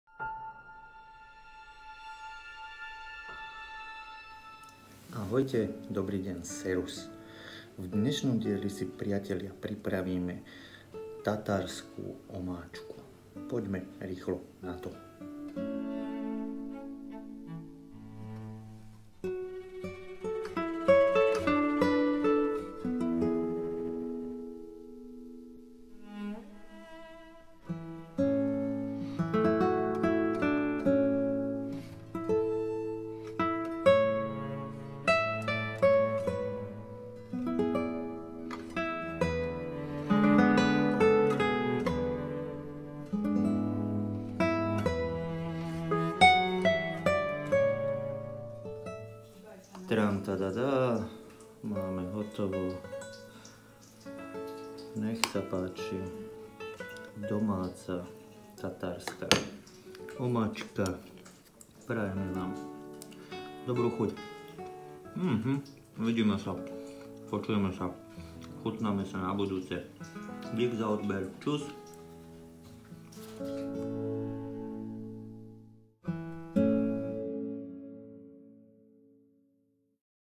# 54 “TARTAR SAUCE” Best of the Best Sauce for French Fries
“TARTAR SAUCE” Best of the Best Sauce for French Fries
Ingredients:
Mayonnaise
Onion
Pickled Gerkins
Salt
Black Ground Pepper

Dobrú chuť!!!
Bon appetite!!!
Guter Geschmack !!!
Dobry smak !!!